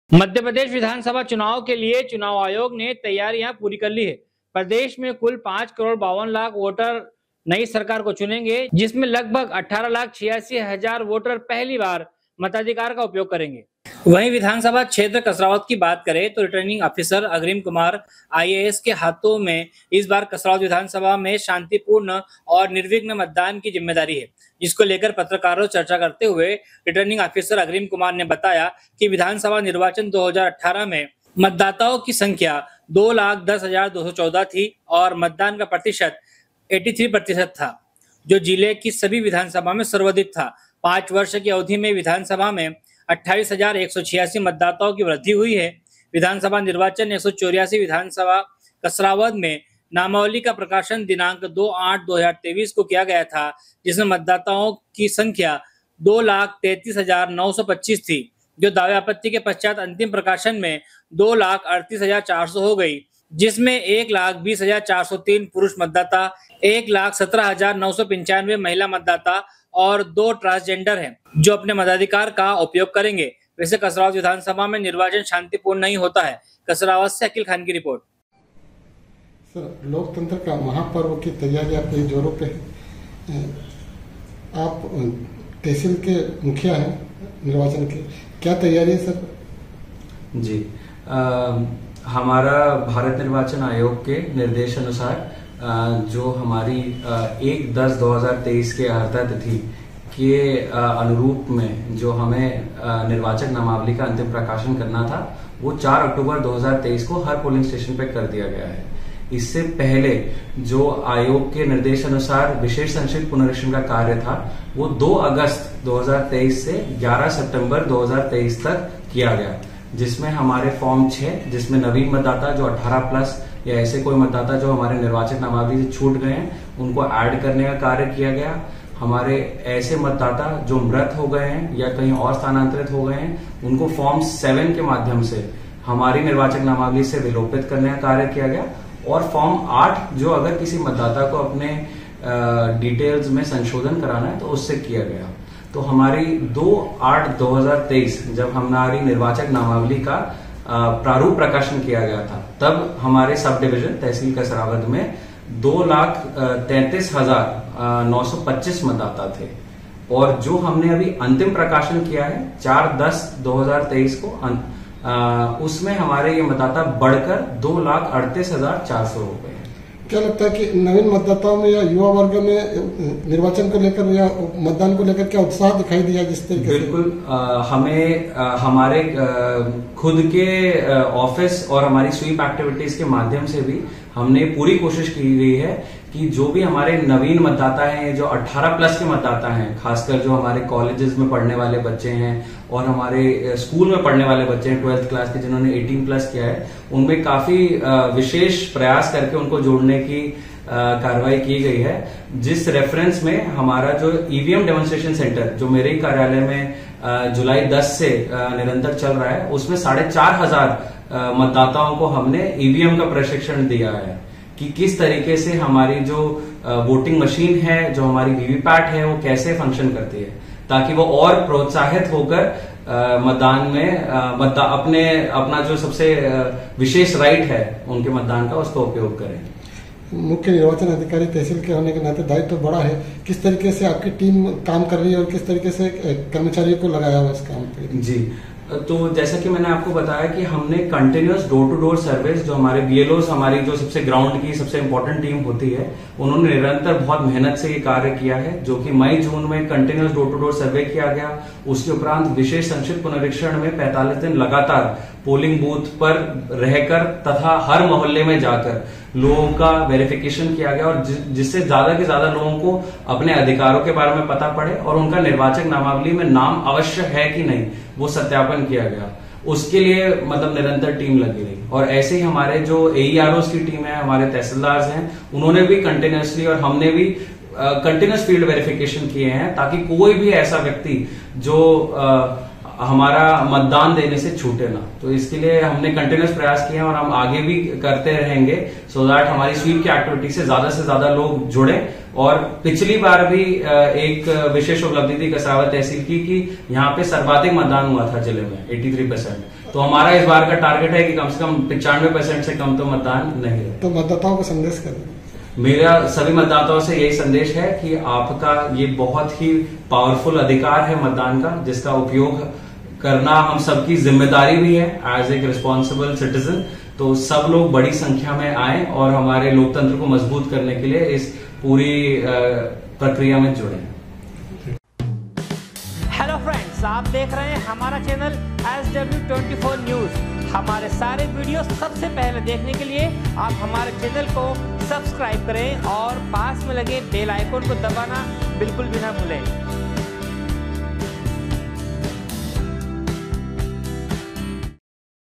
मध्य प्रदेश विधानसभा चुनावों के लिए चुनाव आयोग ने तैयारियां पूरी कर ली हैं। प्रदेश में कुल 5.52 करोड़
मध्य प्रदेश विधानसभा चुनावों के लिए चुनाव आयोग ने तैयारियां पूरी कर ली हैं। प्रदेश में कुल 5.52 करोड़ वोटर नई सरकार चुनेंगे। 18.86 लाख वोटर पहली बार मताधिकार का प्रयोग करेंगे। कसरावद से अकिल खान की रिपोर्ट।